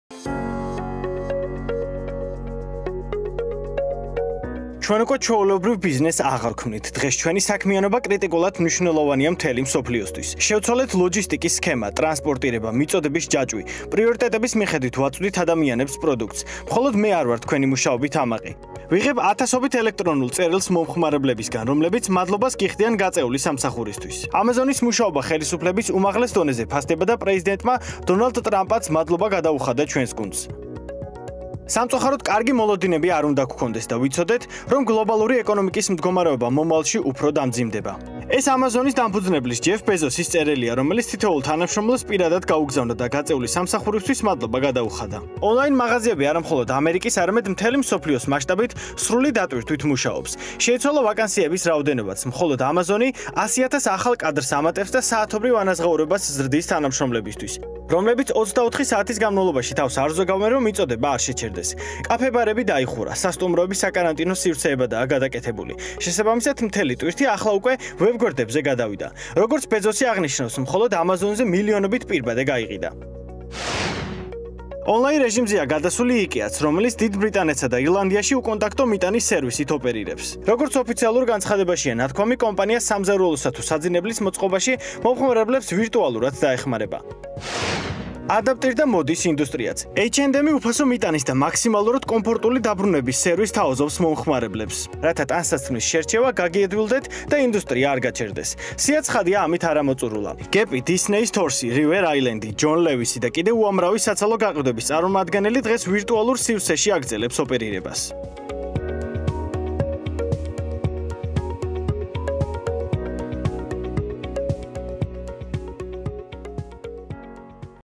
როგორ შეცვალა კორონავირუსმა Online შოპინგის ინდუსტრია?
ონლაინ ვაჭრობა კორონა ვირუსის გამო განსაკუთრებთ ისეთ ვებ-გვერდებს დაეტყო, როგორიცაა Amazon. ის მთელი მსოფლიოს მასშტაბით ემსახურება  მომხმარებლებს, 100 000 ახალ თანაშრომელს ამატებს და სრულად გადასულია 24 საათიან რეჟიმზე. საკმაოდ ემოციურიც გამოდგა ჯეფ ბეზოსის მიერ დაწერილი წერილი, რომელიც თანამშრომლებს გაწეული სამსახურისთვის დიდ მადლობას უხდის. არც ტრამპი ჩამორჩა მოვლენებს და ამაგი მანაც დაუფასა ამერიკელებს და არამხოლოდ მათ.